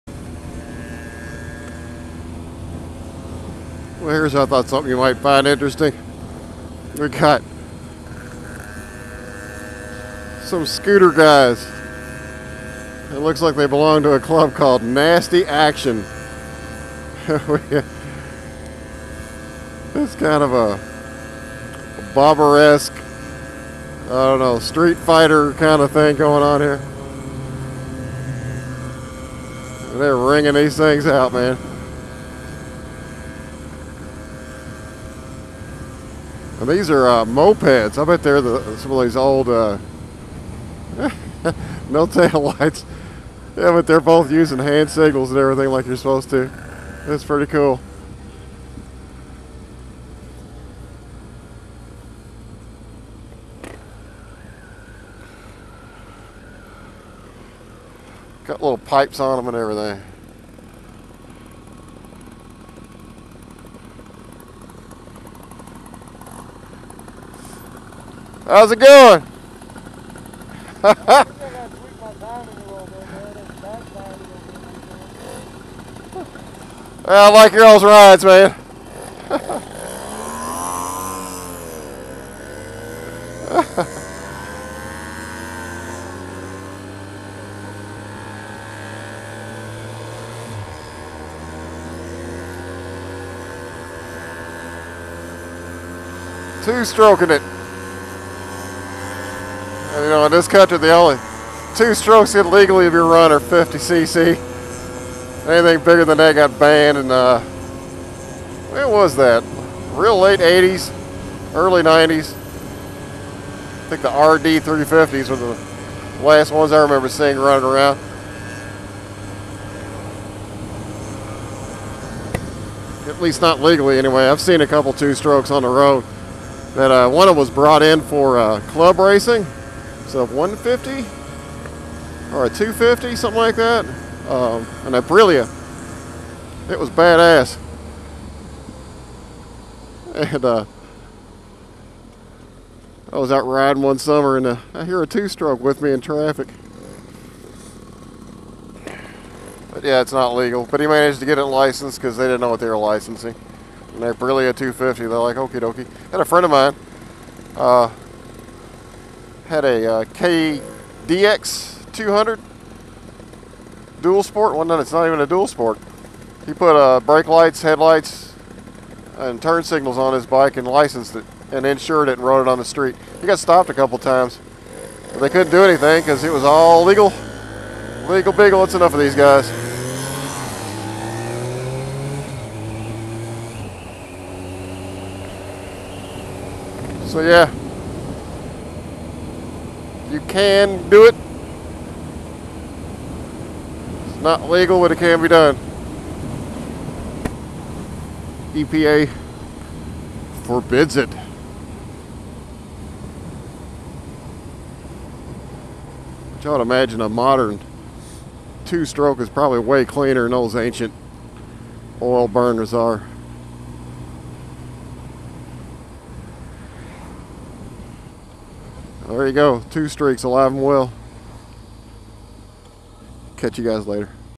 ZRX Diaries: Moped Hooligans
I run across two members of a notorious moped gang on their tricked out mopeds.